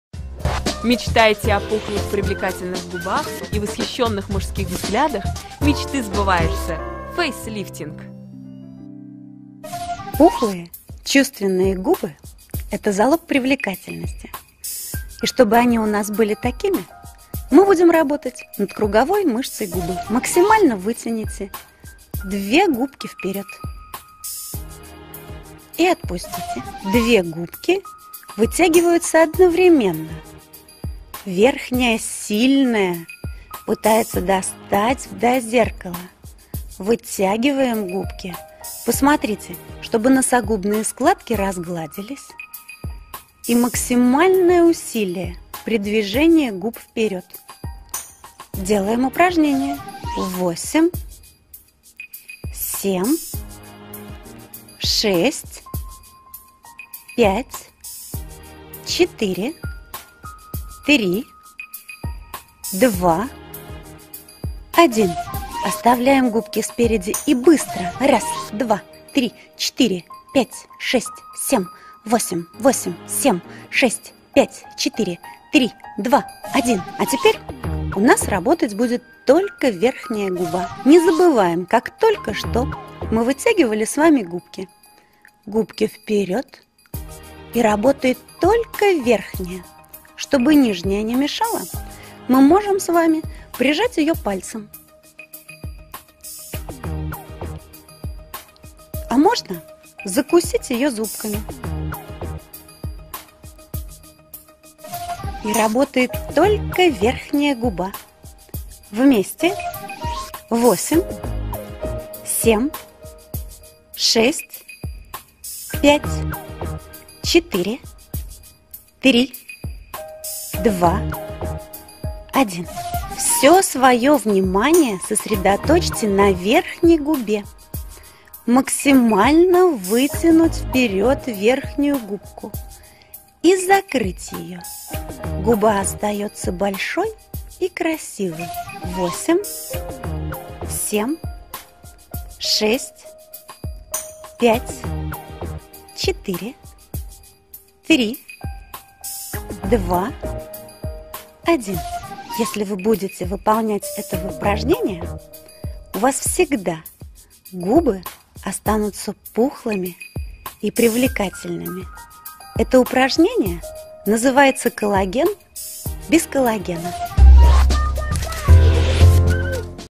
Упражнения для лица Губы
Пластическая операция?! Нет! Зарядка для лица! По уникальной методике Галины Дубининой всего за несколько минут в день можно повернуть время вспять! Оригинальный комплекс упражнений против складок на шее, мешков под глазами и морщин на лбу; точечный массаж ушей, бровей и головы; бодифлекс и зарядка для глаз, гимнастика для пробуждения и маленькие секреты, как привести себя в порядок перед походом на вечеринку. Результат налицо? Нет, результат на лице! Никаких чудес -- просто FaceЛифтинг!